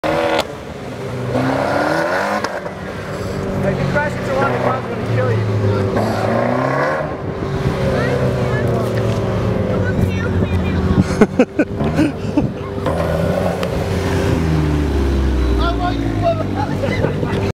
Fiat Abarth
Fiat Abarth showing off trying to make donuts .